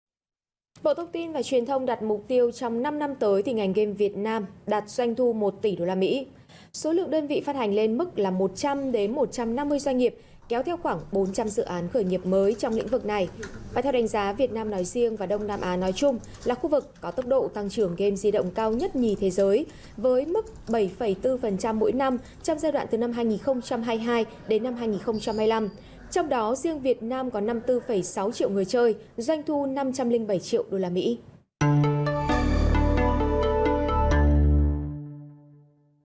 Ngành game Việt Nam phấn đấu doanh thu 1 tỉ USD - VNEWS
VNEWS - Ông Lê Quang Tự Do, Cục trưởng Phát thanh, truyền hình và thông tin điện tử, cho biết Bộ Thông tin và Truyền thông đặt mục tiêu trong 5 năm tới ngành Việt Nam đạt doanh thu 1 tỉ USD.

Mời quý vị và các bạn xem thêm: Bất ngờ về vật thể bí ẩn gần hiện trường vụ nổ đường ống dẫn khí đốt Nord Stream